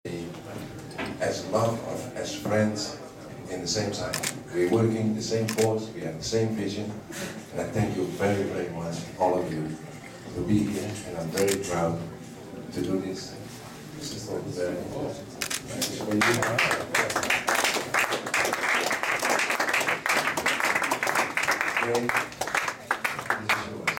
Opening renewed Hash Marihuana and Hemp Museum Amsterdam
Ben Dronkers reveals a framed photo "stop the war on drugs". Global Commission on Drug Policy and is dedicated to Thorvald Stoltenberg